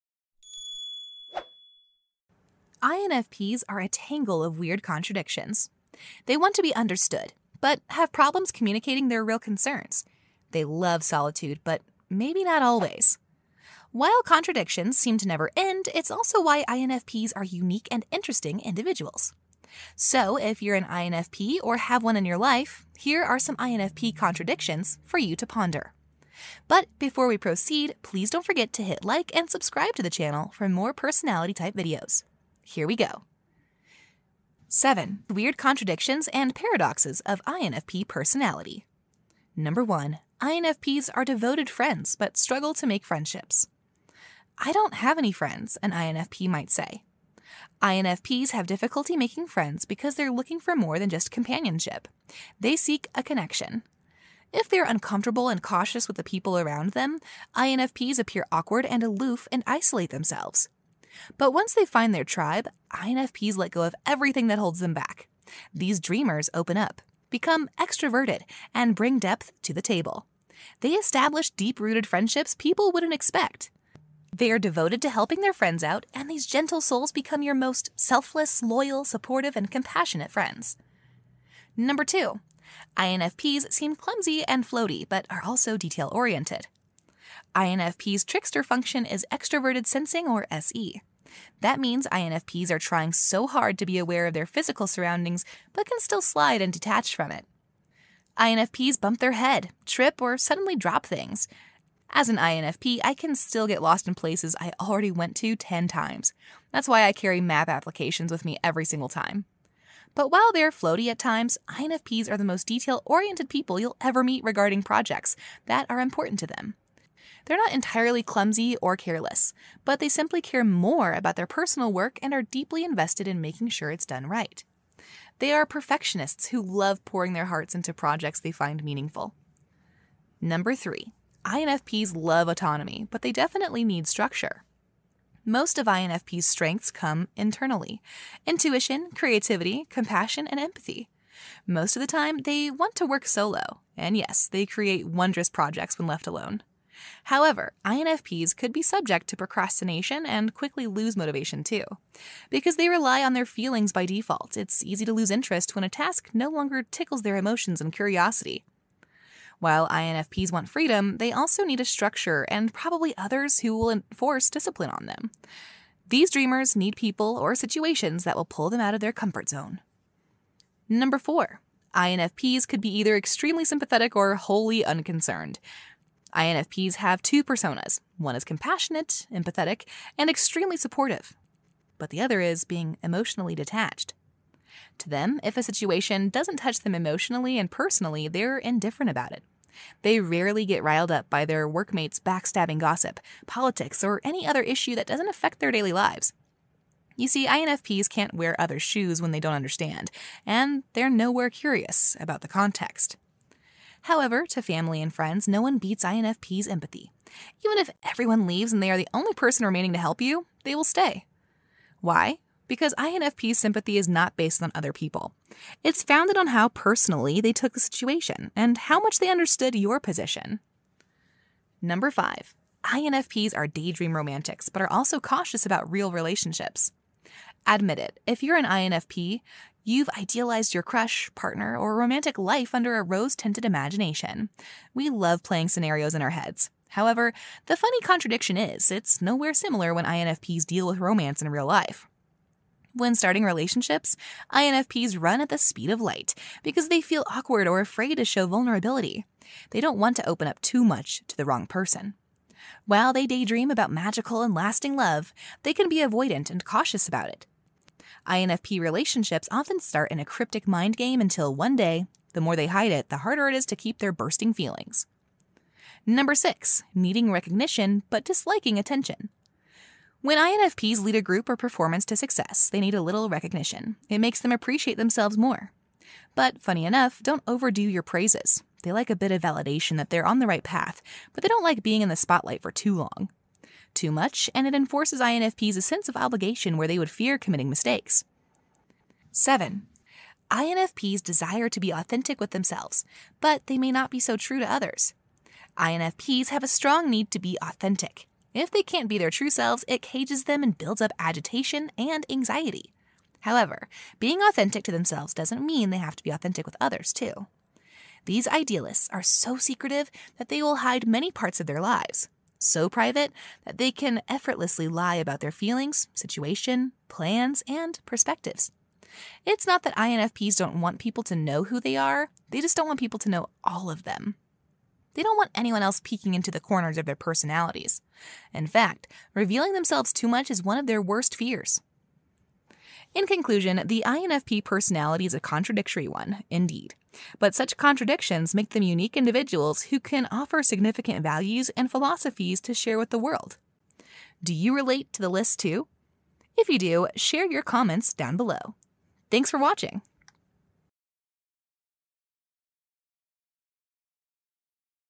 7 WEIRD Contradictions of INFP Personality
INFP paradoxes?

See, we’re fiercely independent, but we also deeply value relationships with others. We want to be understood, but have problems communicating our real concerns. We love our solitude—but maybe not always.

Goodness, I think our contradictions never end! But it’s also why INFPs are unique and interesting individuals. It’s the reason why people can’t easily pin us down—but that’s what makes us mystifying, right? Right.

So, if you’re an INFP (or have one in your life), here are some of INFP contradictions for you to ponder.

If my blogs and videos are of some help to you, you might also want to check out my e-book, "Not Lazy, Just INFP"
Thanks! :)

Animation: Garubin